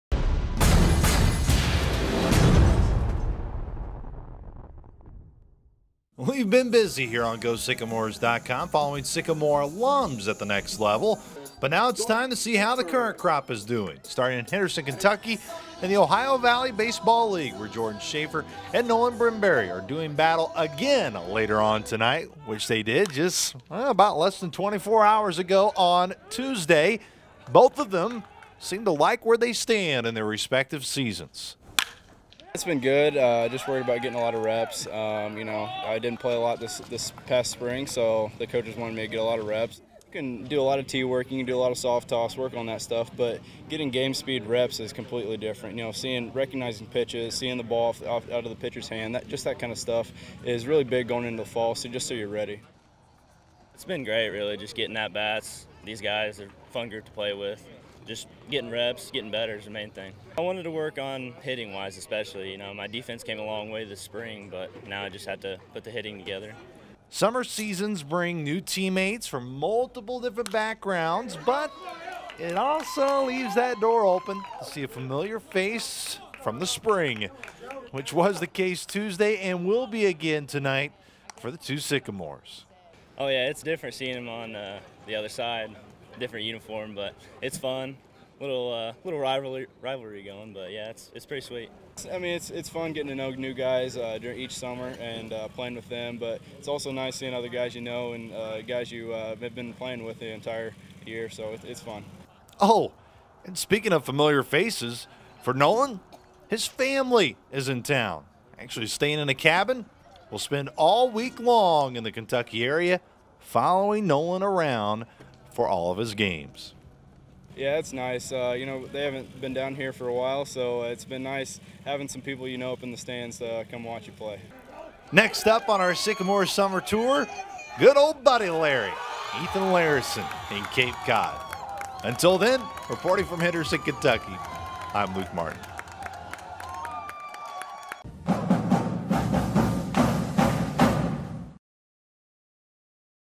Baseball Summer Update: Jordan Schaffer and Nolan Brimbury
Luke Martin begins his summer Tour of going around and catching up with current Sycamore players in summer league baseball!